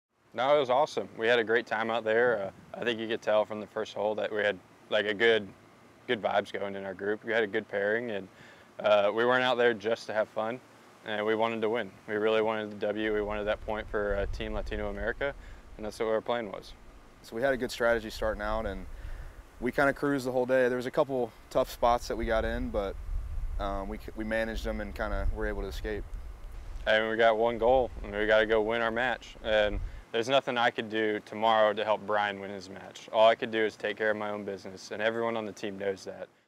Hank Lebioda and Brian Richey interview after Day Two of the Aruba Cup
Following Day Two of the 2017 Aruba Cup, Hank Lebioda and Brian Richey talk about their foursomes victory and the challenges ahead on the final day at Tierra del Sol Resort and Golf.